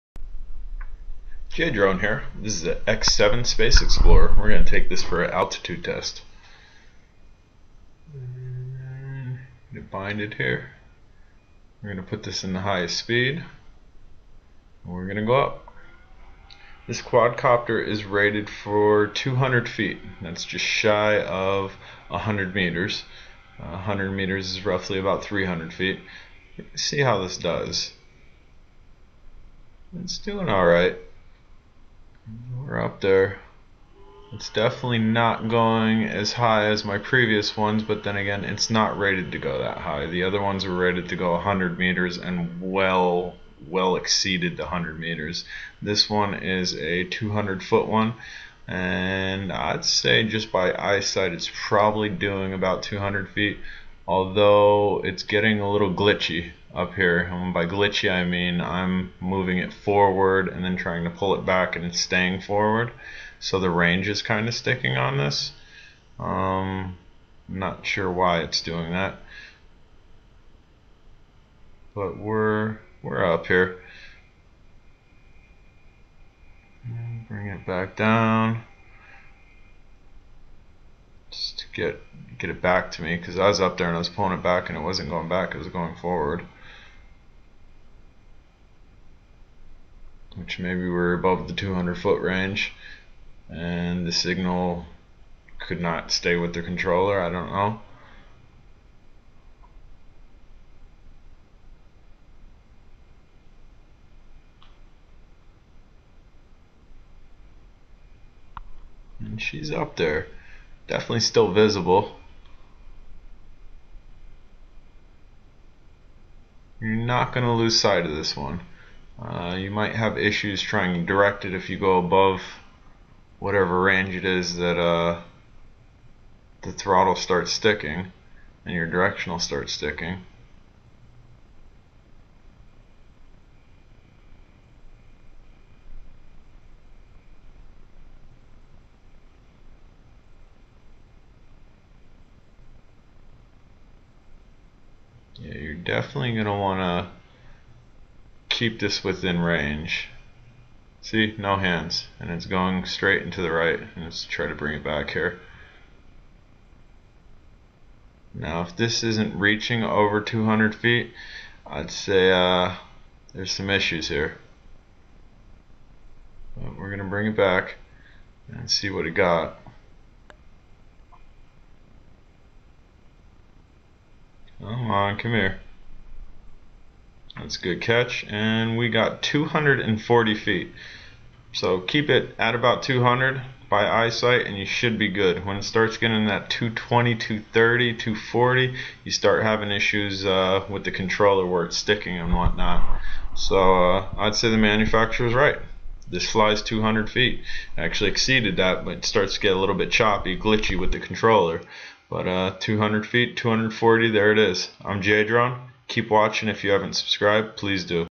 X7 Space Explorer Drone Altitude Test 240ft Jolly Logic AltimeterOne
This is the X7 Space Explorer Drone Altitude Test reached 240ft using the Jolly Logic AltimeterOne. Gets a bit glitchy above that 200 mark